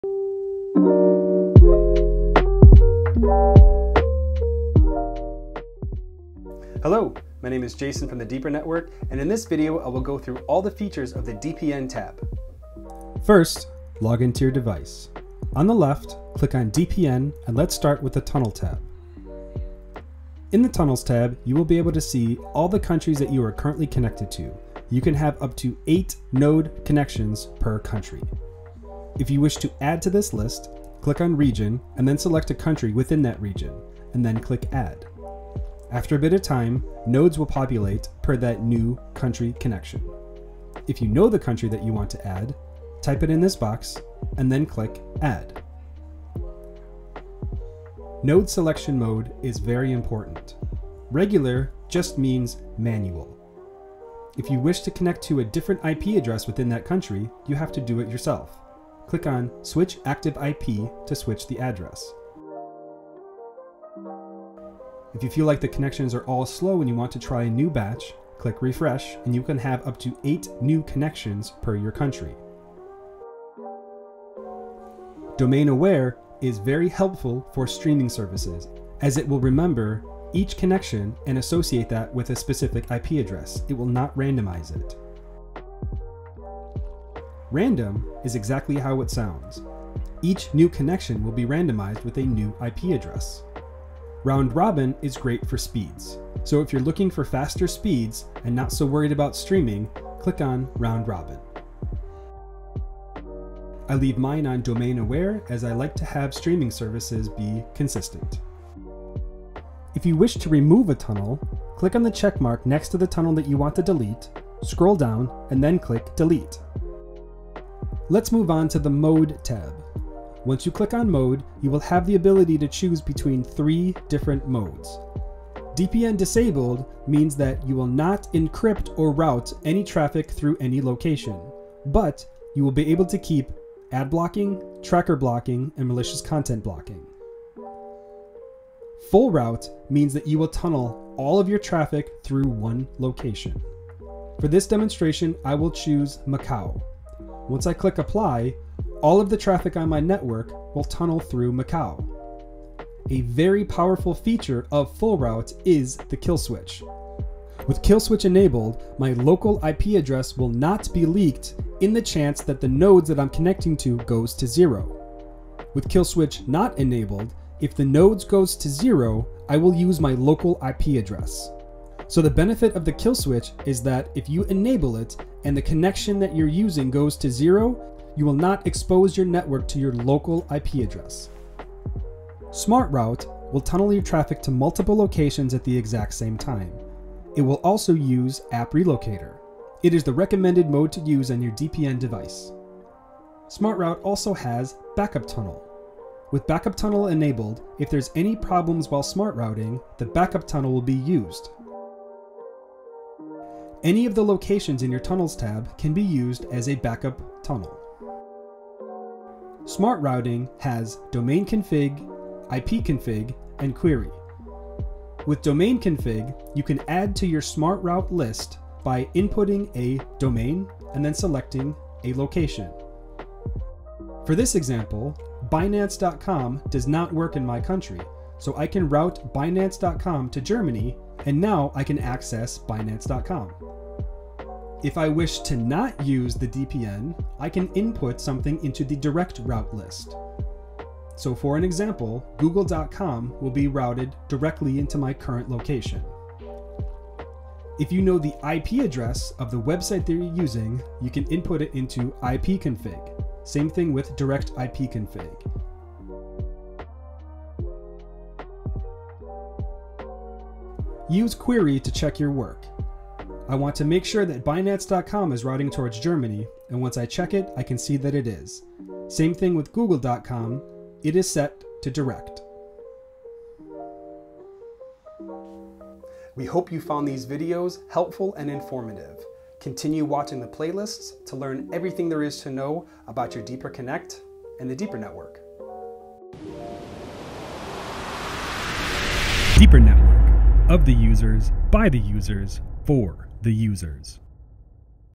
DPN Tab
Welcome Deepernauts!

This video will walk you through everything there is to know about the DPN Tab! It's great for anyone new to AtomOS or for users to brush up on their routing!

GitHub.com/skaterjuice/deeperblacklist